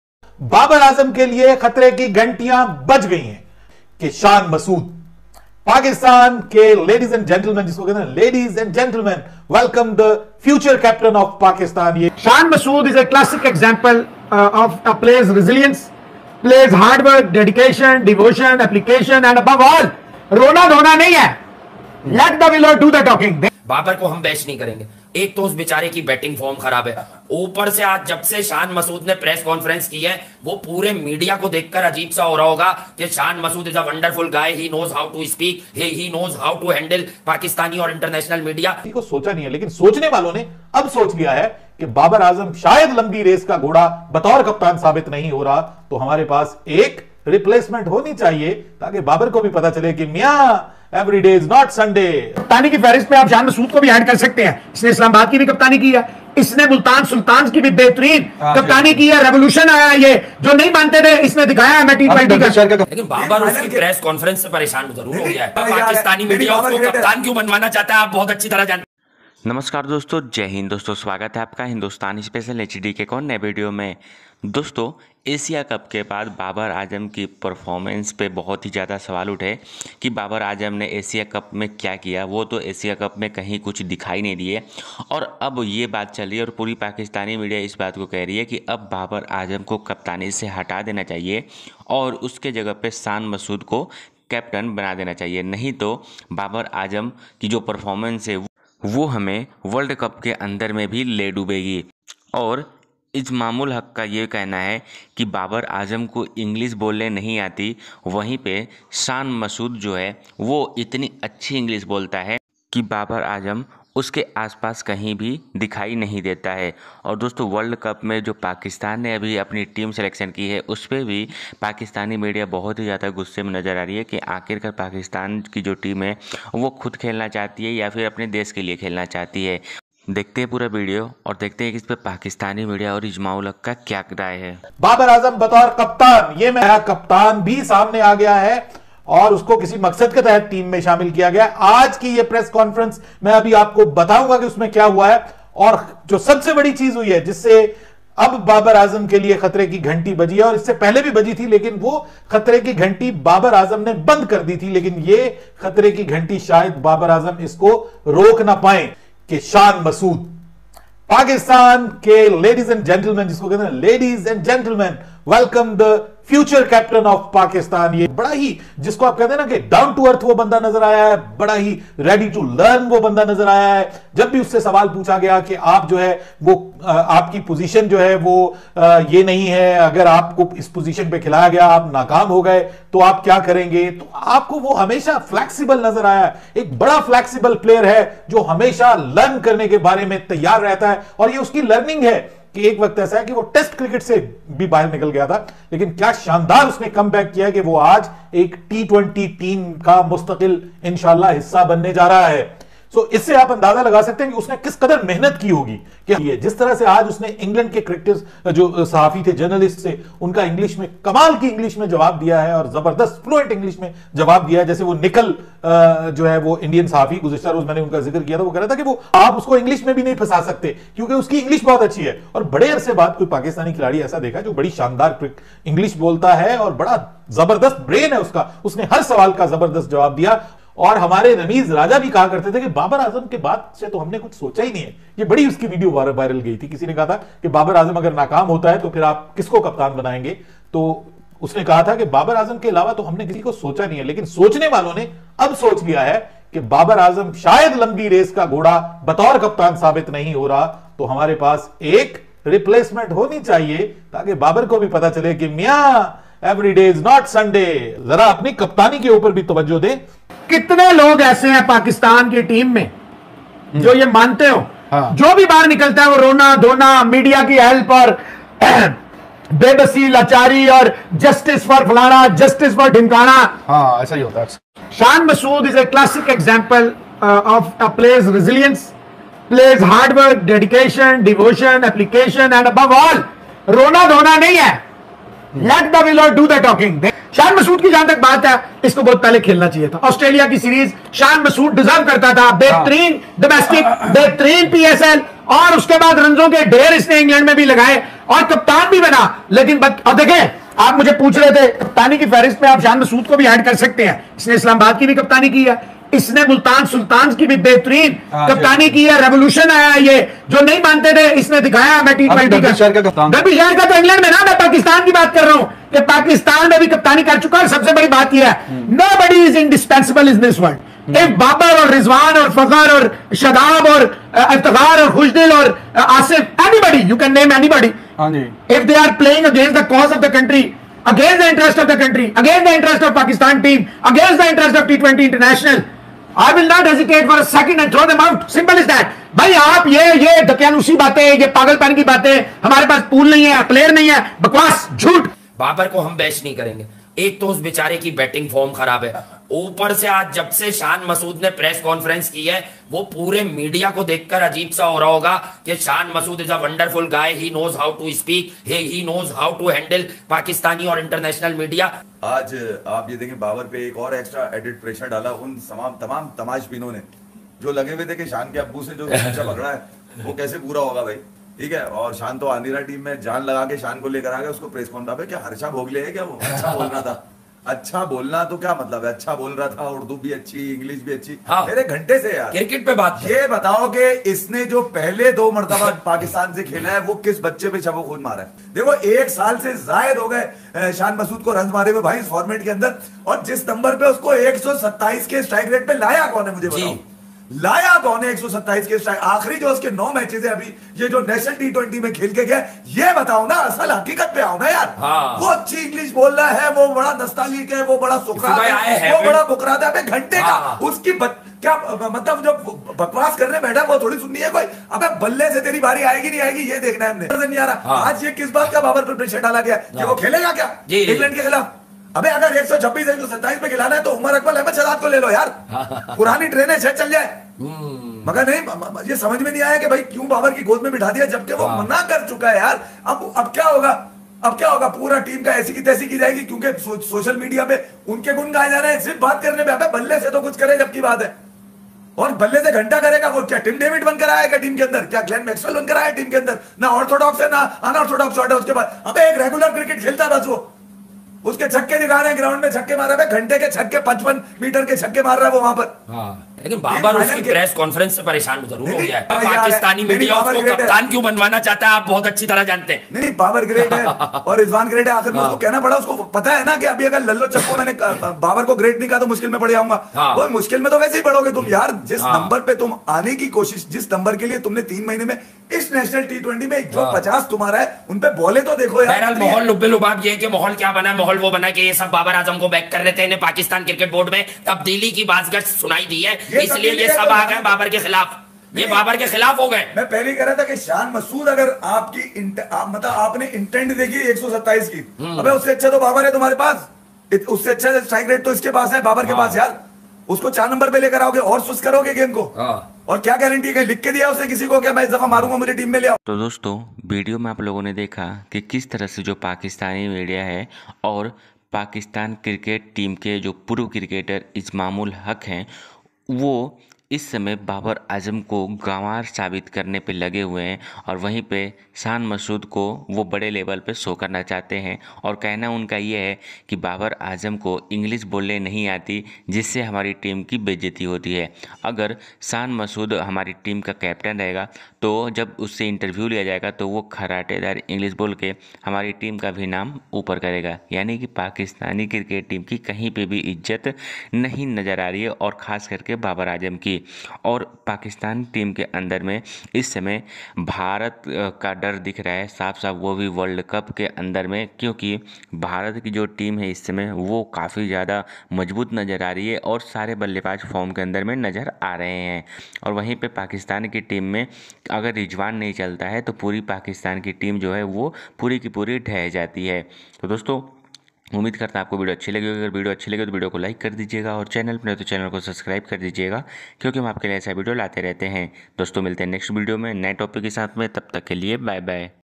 Inzamamul Haque said, take away the captaincy of Babar, Babar is Gawar Pak Media Pak reaction
Inzamamul Haque said, take away the captaincy of Babar, Babar is Gawar Pak Media On Would Cup 2022

Hindustan Special HD